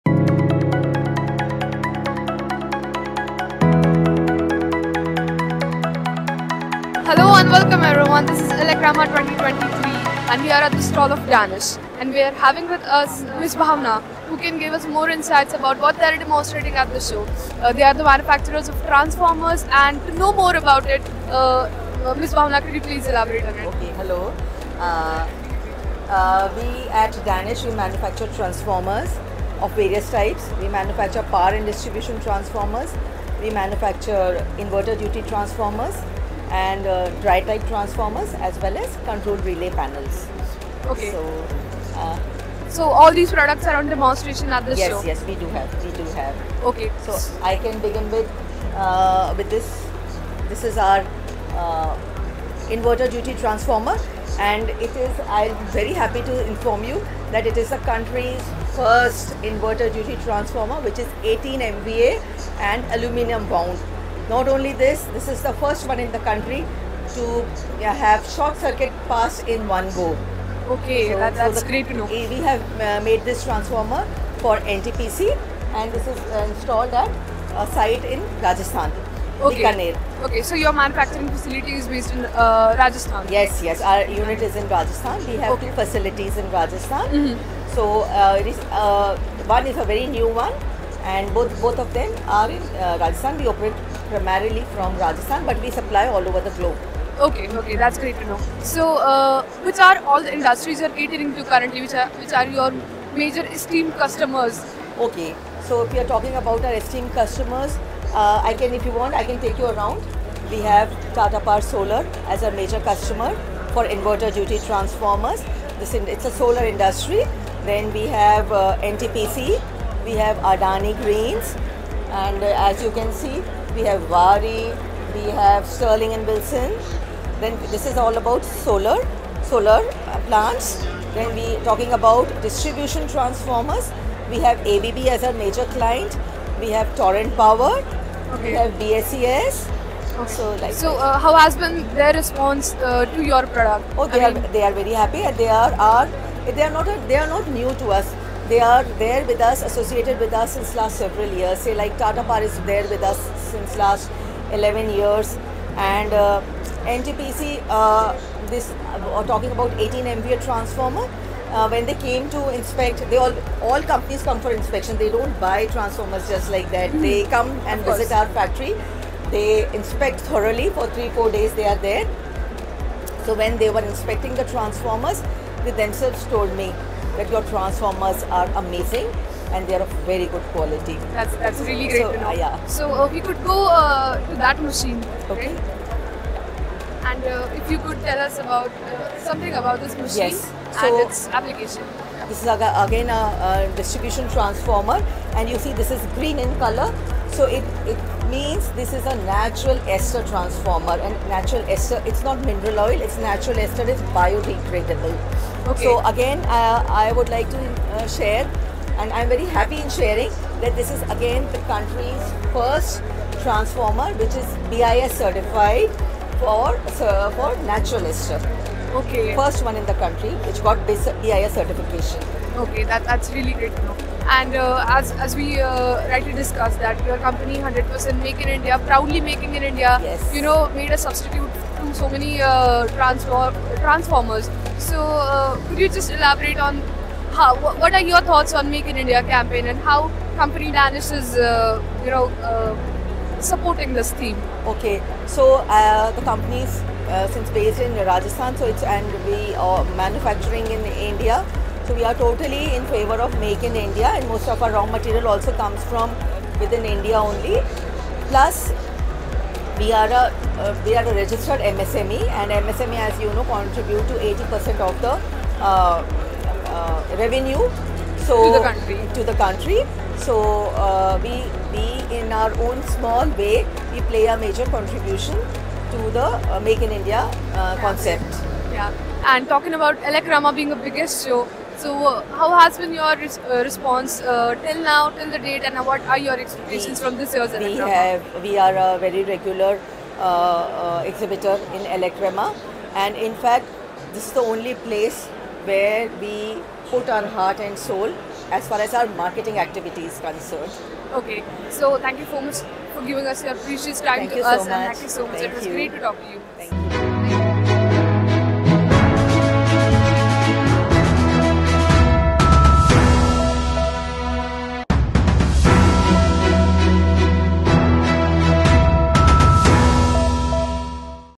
Transforming Power with Innovative Power Solutions from Danish Pvt Ltd
Danish Private Limited, established in 1985, is a reputable manufacturer and exporter of power equipment in India. They take pride in producing high-quality oil and dry type power and distribution transformers, control and relay panels, and providing substation automation services.  Bhavana Saxena Assistant General Manager at Danish Private Limited shared some insights about the company at Elecrama 2023.

Danish Pvt Ltd is operated from two manufacturing units in Rajasthan and has major clients in the solar and distribution transformer sectors, including Tata Power Solar, NTPC, AdaniGreens, in solar transformers and Torrent Power, ABS, and BSES in distribution transformers. The company has also developed the country's first DIS certified natural ester transformer, which is biodegradable.

As an MSME, Danish Pvt Ltd contributes to 80 percent of India's revenue generation done by MSMEs, supporting the Make in India initiative. The company purchases raw materials from within India, further contributing to this initiative.

Danish Pvt Ltd has been a regular participant in Elecrama, participated the 2023 exhibition, to promote its products effectively.